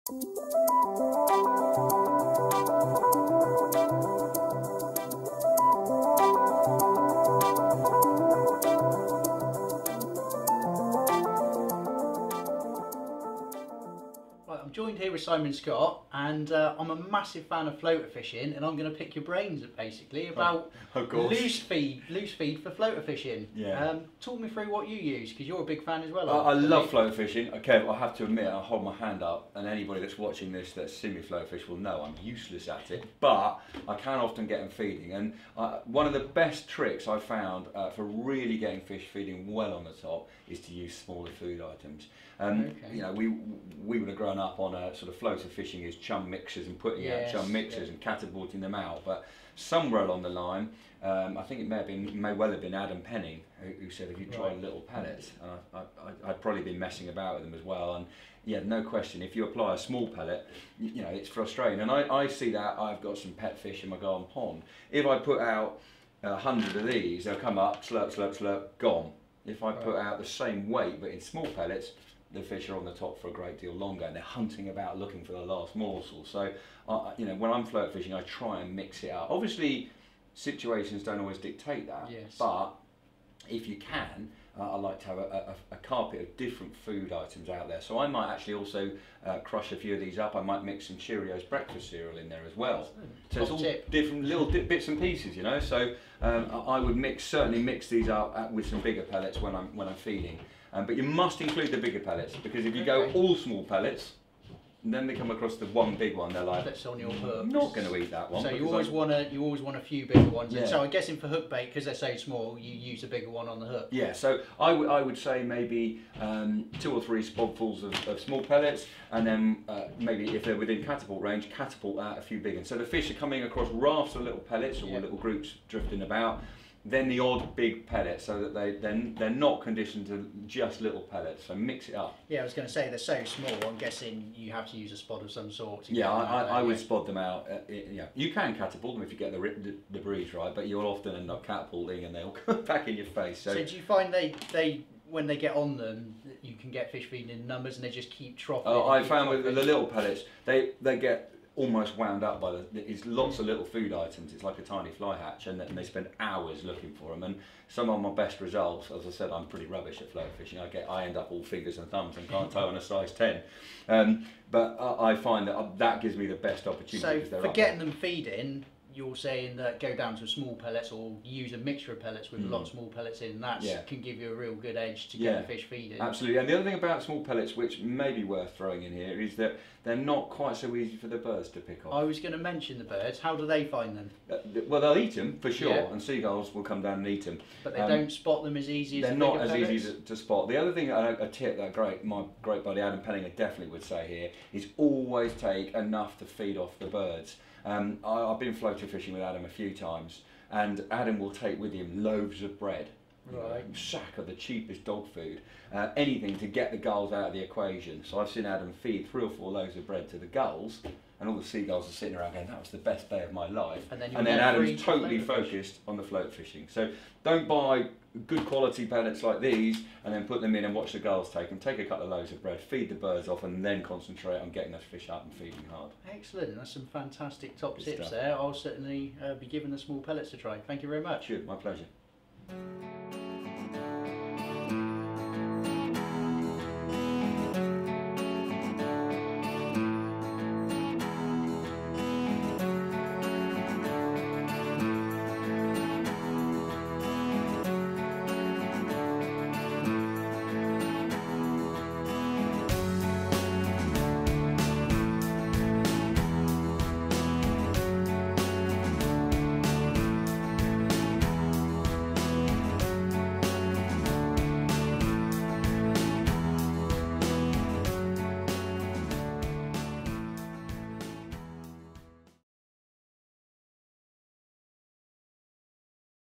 Floater Fishing For Carp With Korda Man Simon Scott
Korda Consultant Simon Scott chats to Kev about his approach for Floater Fishing a must have approach for Carp Fishing in the Summer. Simon gives his top tips for feeding whilst Floater Fishing massively important for success whilst fishing